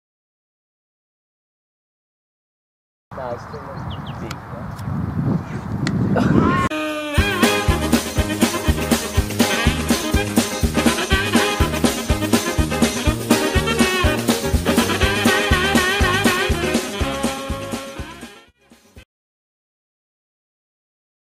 Horrible fielding - 3rd Grade vs Bayswater-Morley
Last ball of the Willetton Innings, Josh Kuser on strike, ball finished 10m from the bat, we ran 4! Ben Witschge praying Baysie to just hold the ball!